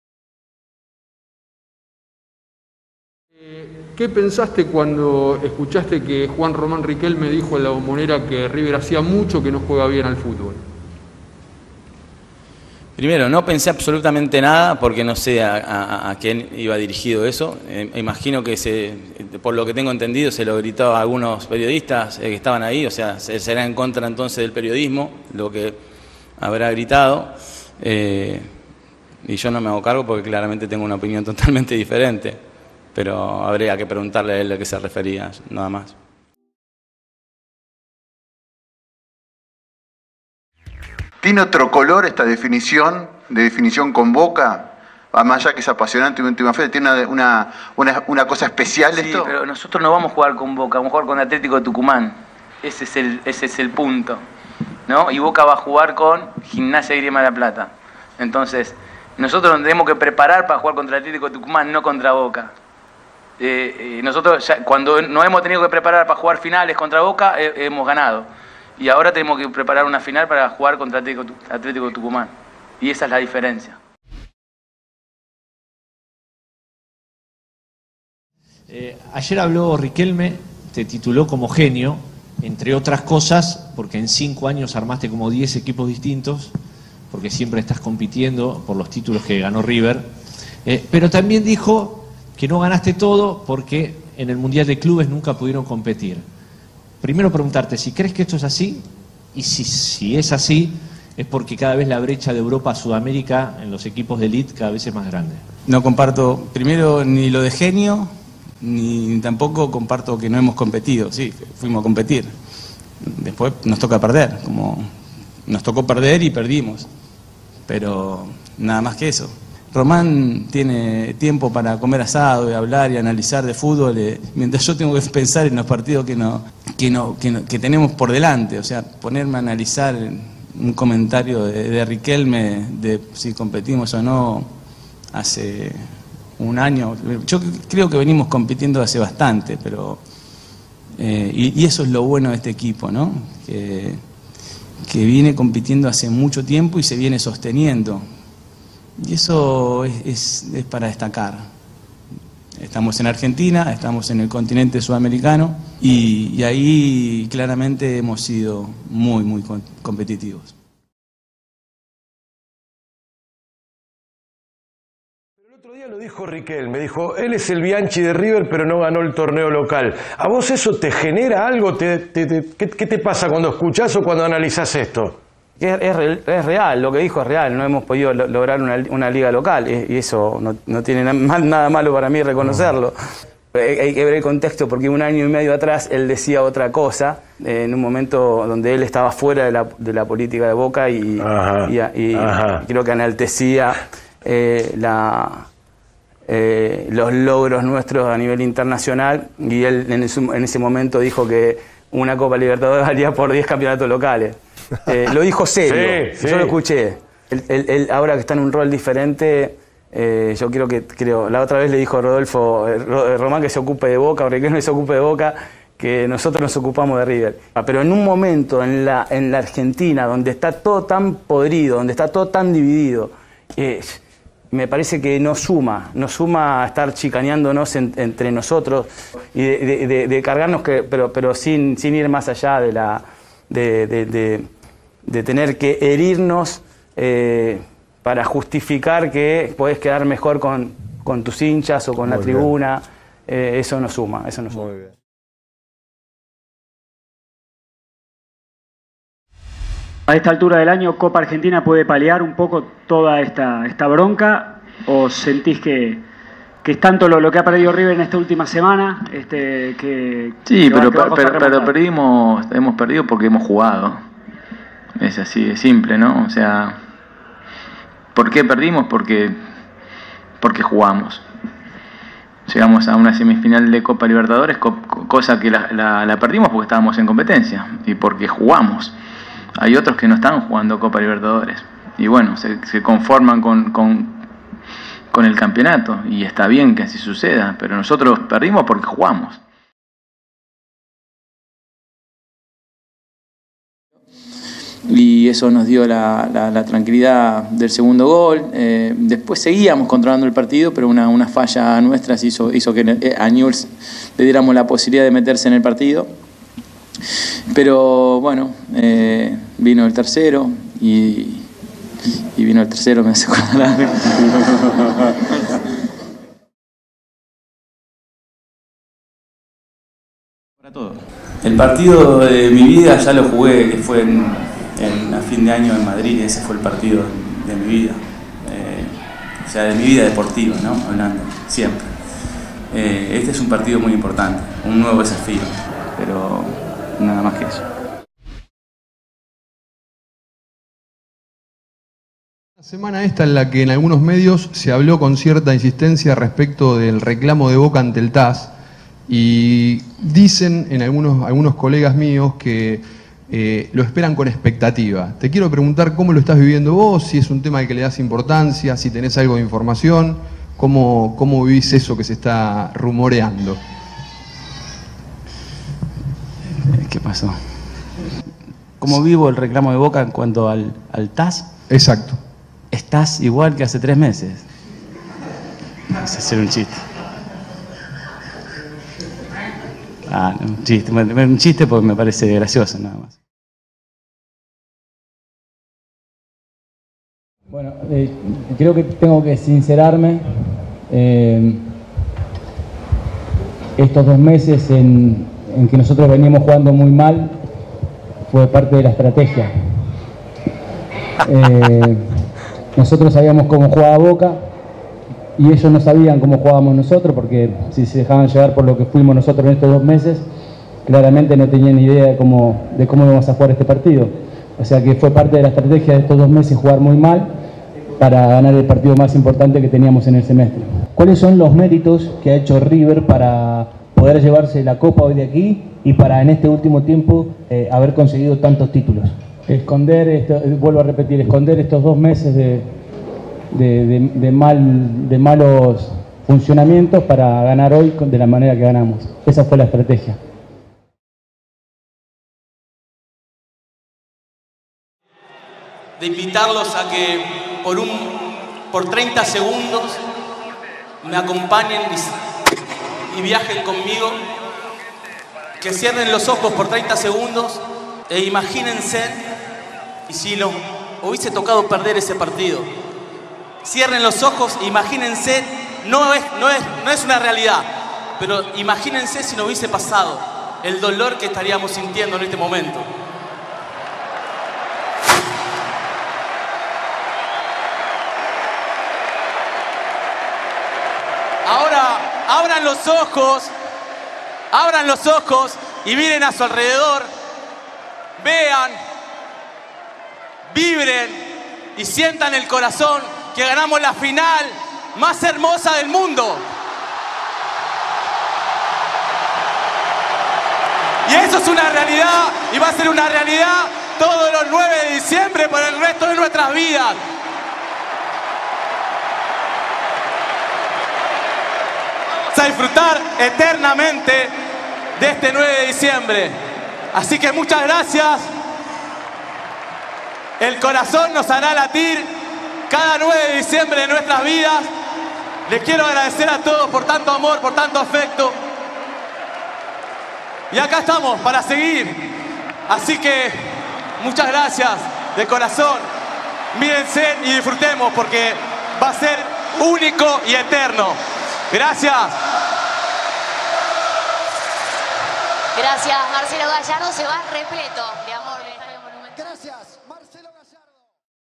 LAS MEJORES CHICANAS DE GALLARDO A BOCA
Hola millos! Hoy les traigo un nuevo video, esta vez de las 10 mejores chicanas de Marcelo Gallardo al ex club. Espero que les guste!